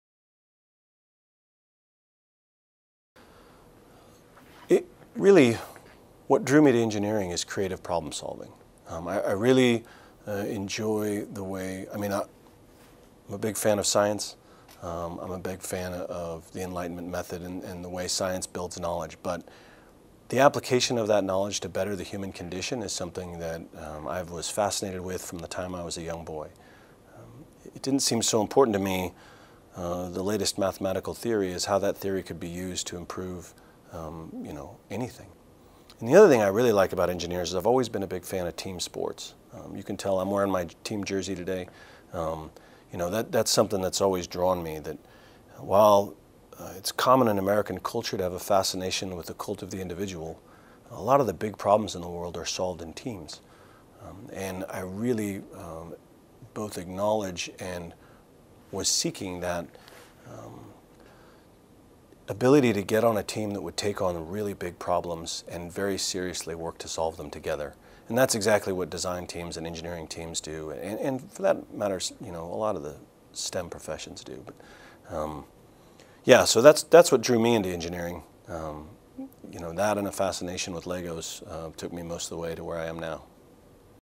Engineering in the Military - The Appeal of an Engineering Career
U.S. Army Lt. Col. Xander Bullock explains why he chose a career in engineering.